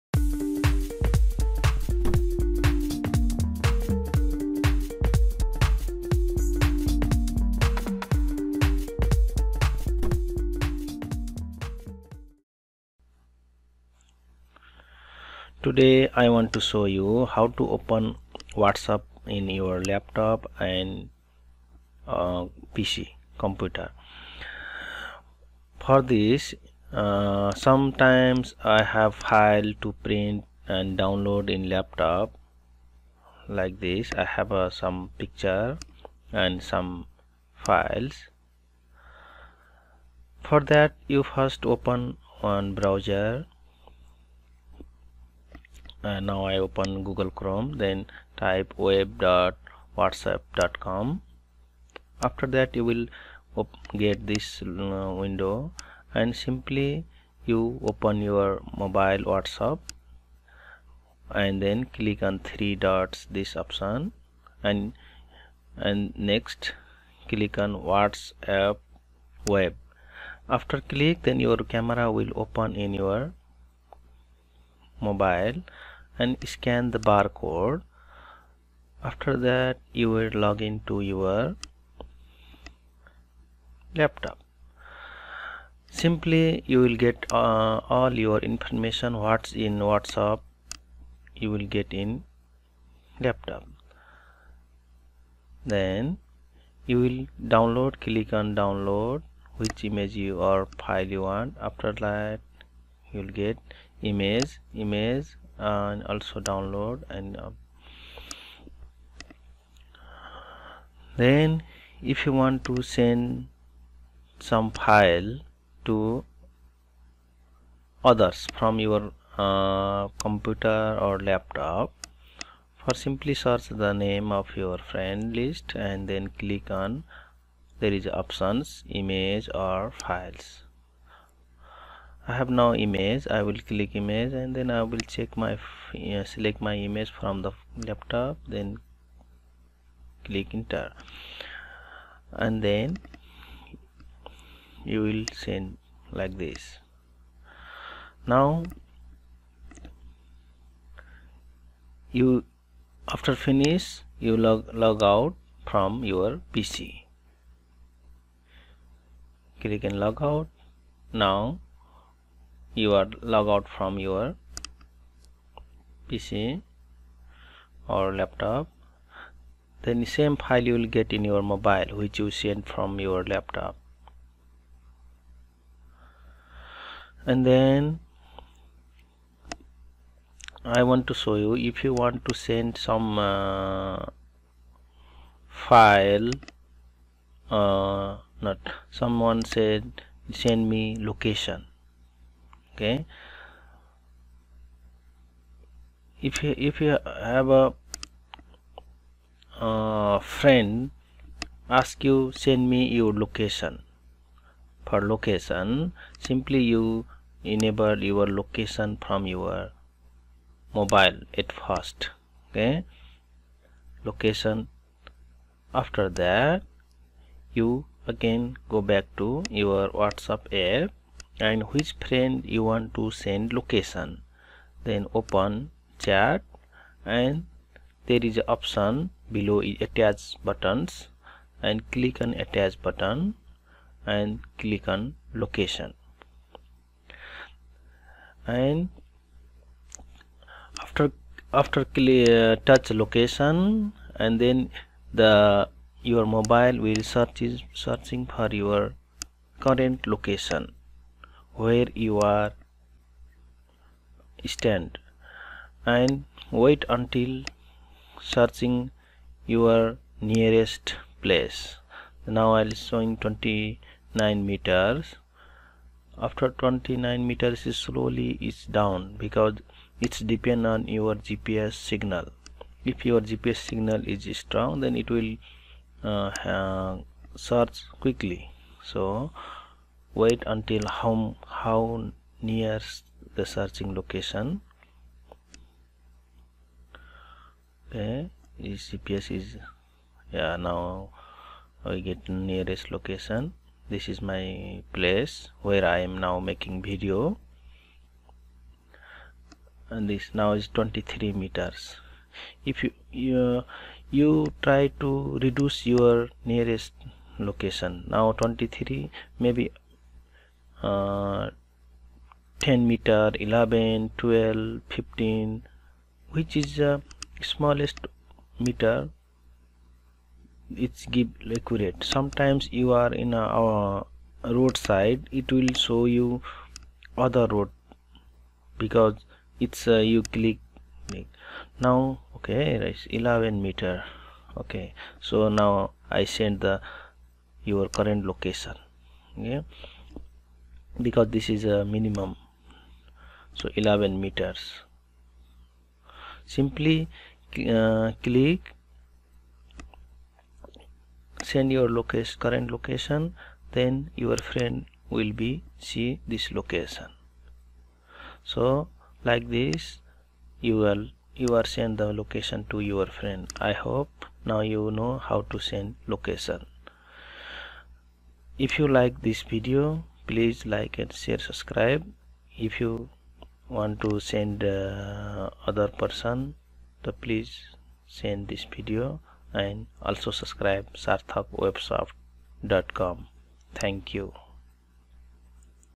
How to open WhatsApp on Laptop or Computer and Send Location?
How to use WhatsApp in laptop? How to send location? Sarthak WebSoft.